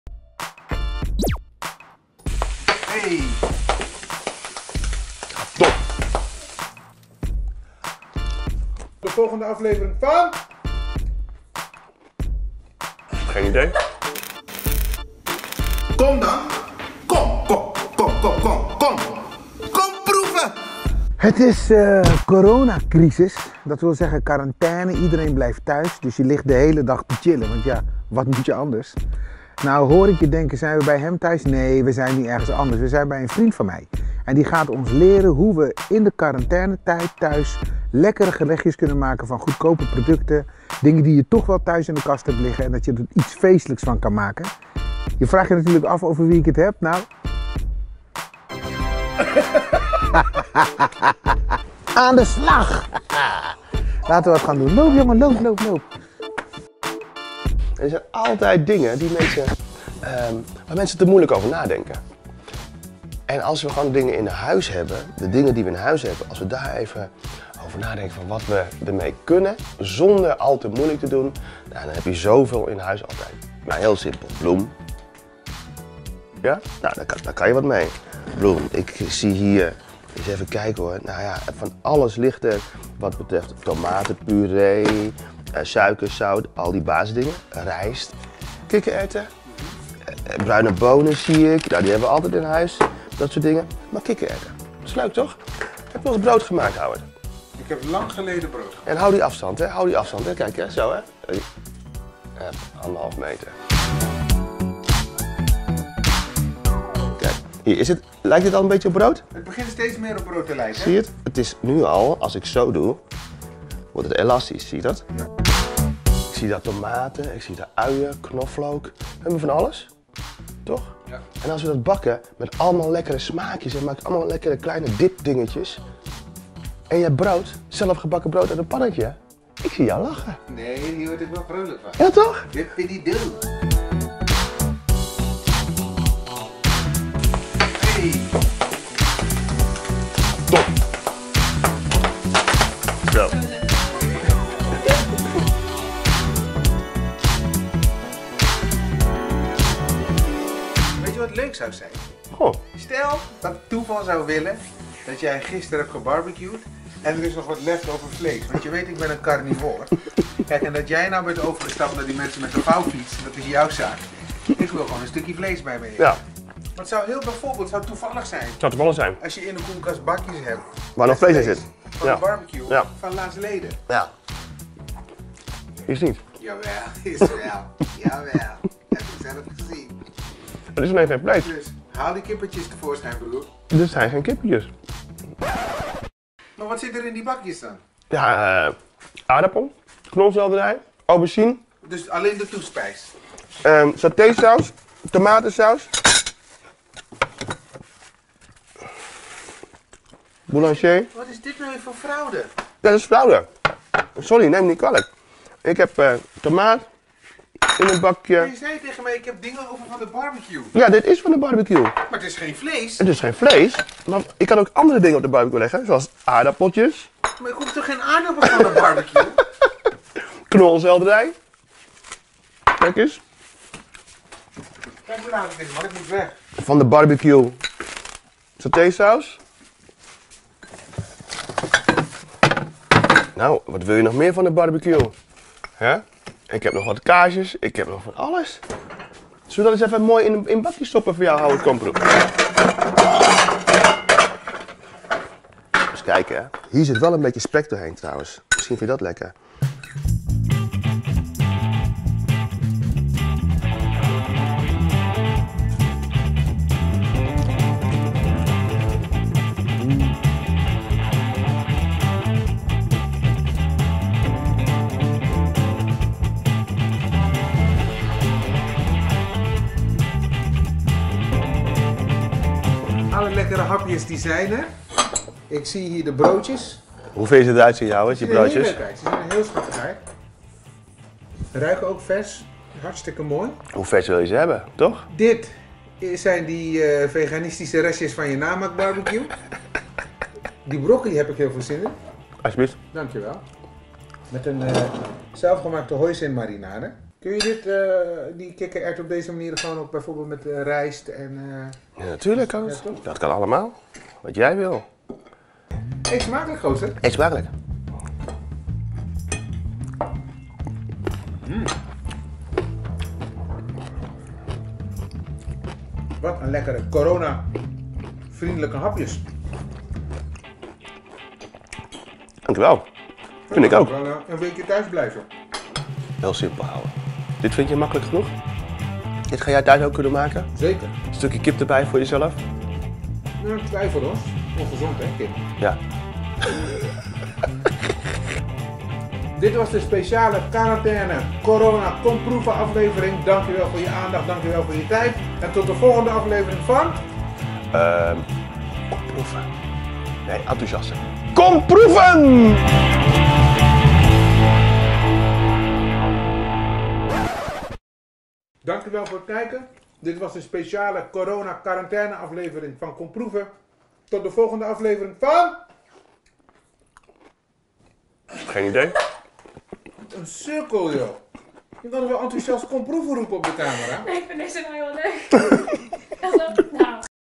KOMPROEVEN 'CORONA SPECIAL': krijgt Ramon Beuk mij aan het twijfelen over KIP!?
KOMPROEVEN: vandaag gaan we brood maken met de enige echte Ramon Beuk! We gaan niet zomaar bammetjes maken natuurlijk, we duiken in de kast en gebruiken van alles wat we kunnen vinden. En de vraag is: krijgt Ramon mij aan het twijfelen over kip?? Kijk het hier, weg corona-verveling, samen aan de (be)slag! Eet smakelijk allemaal, en dank je wel Ramon!

Volg Howard Komproe ook op:
Instagram & Twitter: @howardkomproe

Credits:
Presentatie/productie: Howard Komproe (@howardkomproe)
Regie/camera/edit: ME Visions Estelle Levinson (@estellelevinson @mevisions)
Kok: Ramon Beuk (@Ramon_beuk)